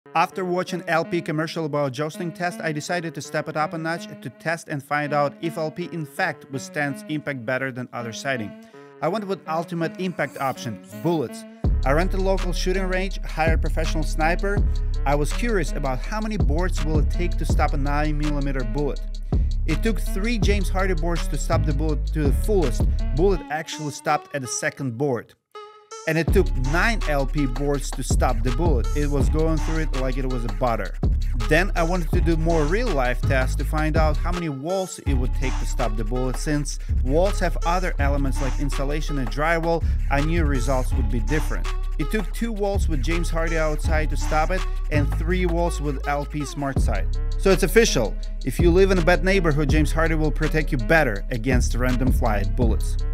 James Hardie VS LP SmartSide Bullet Test
In this video, I'm comparing the JamesHardie VS LP SmartSide Bullet Test. I'm giving you my honest opinion on which one is better. Which one do you think is the better siding choice for your home?